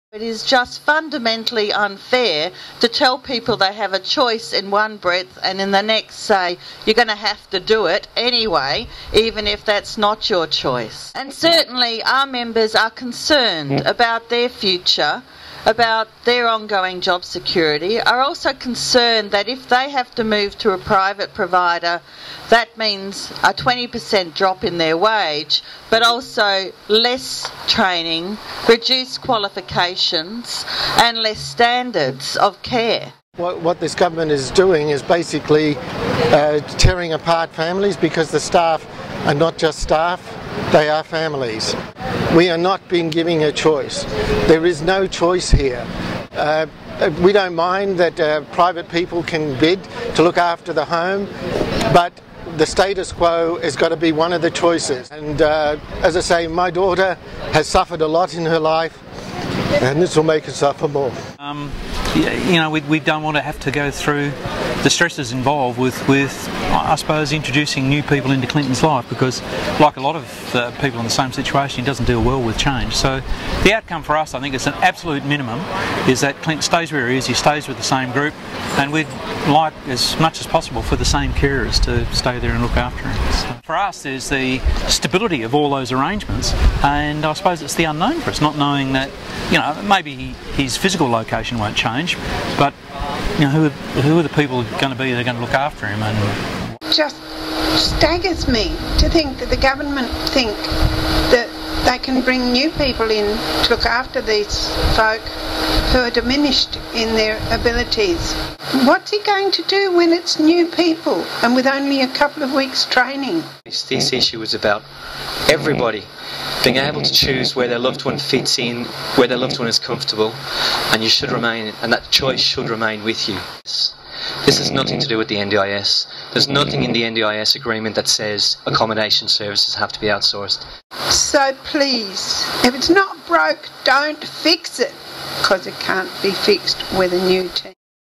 Families take privatisation concerns to parliament
A petition with more than 2000 signatures calling on the Barnett Government to reverse its decision to privatise Disability Services Commission group homes was tabled in State Parliament on Thursday June 26.